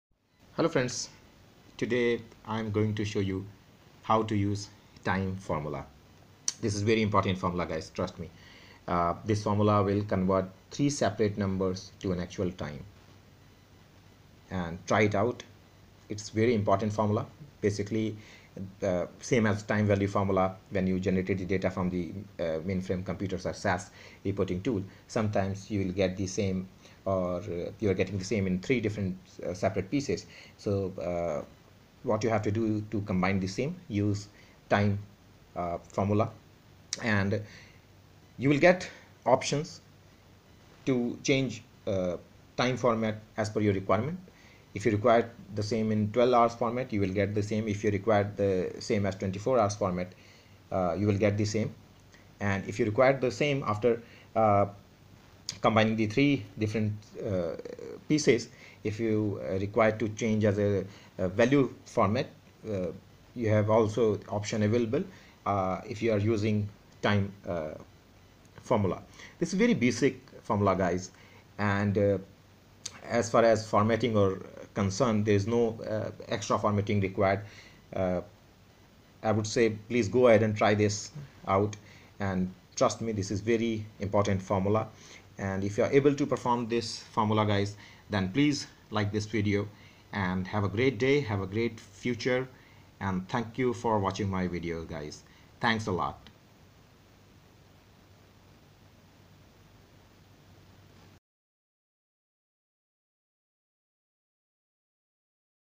“TIME” Excel Formula (In English With Best Examples) How To Apply This Excel Formula
USE OF FORMULA
MEANING: - TIME VALUE FORMULA will show an actual time based on a piece of text which looks LIKE A TIME
This formula is very useful when data is generated from other applications, such as from reporting tool, which converts all values to text after generated from tool

Formatting Required or Not?? : - If you use this formula then The result will be shown as a number representing the time a fraction of the day.
Formatting can be applied for either; 12 or 24 hour clock system (as per requirement)

A

B

C

D

E

F

G

H

M

Q

T